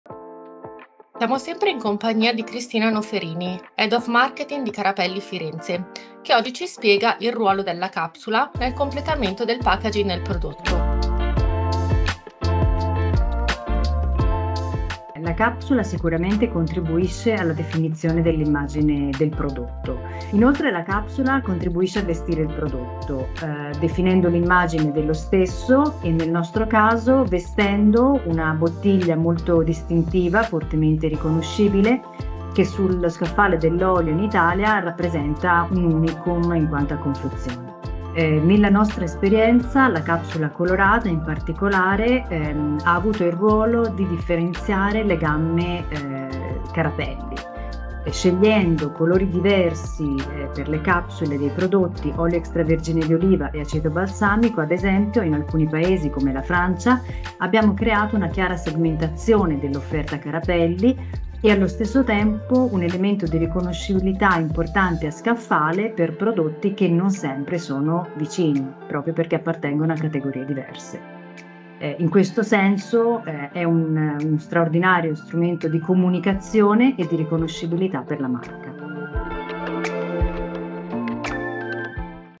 La capsula: un packaging strategico episodio 2 - Carapelli Firenze
🎙️​ La capsula: un packaging strategico
2a puntata – Carapelli Firenze S.p.A.

Insieme alla nostra ospite Cristina Noferini, Head of Marketing di Carapelli Firenze, parliamo del valore della capsula per le bottiglie di oliodoliva e acetobalsamico.

🎙️ The capsule: a strategic packaging
2nd episode - Carapelli Firenze S.p.A.

Together with our guest Cristina Noferini, Head of Marketing of Carapelli Firenze, we talk about the value of the capsule for oliveoil and balsamicvinegar bottles.